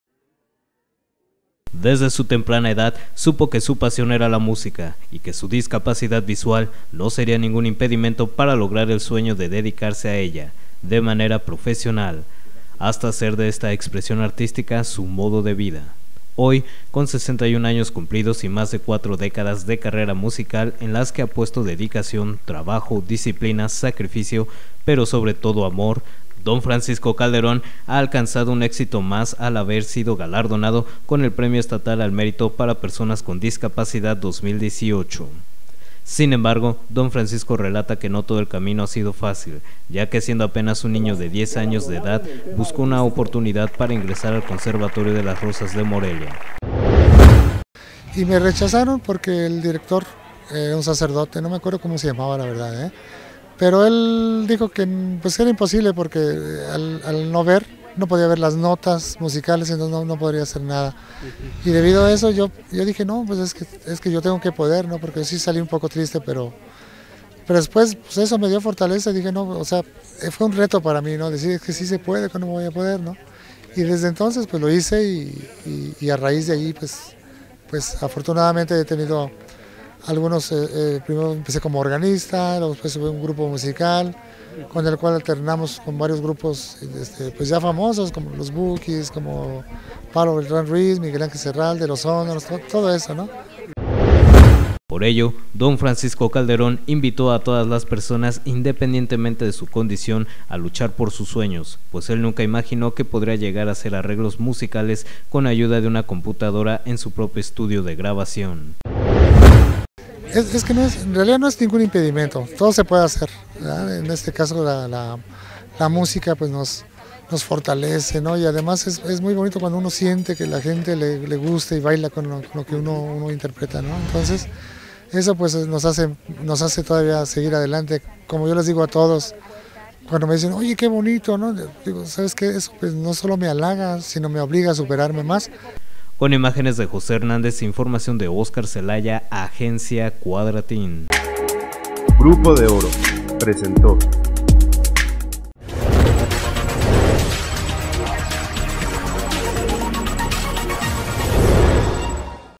Francisco Calderón, historia de éxito rompiendo barreras de discapacidad
MORELIA, Mich., 4 de diciembre de 2018.- Desde temprana edad supo que su pasión era la música y que su discapacidad visual no sería ningún impedimento para lograr el sueño de dedicarse a ella de manera profesional, hasta hacer de esta expresión artística su modo de vida.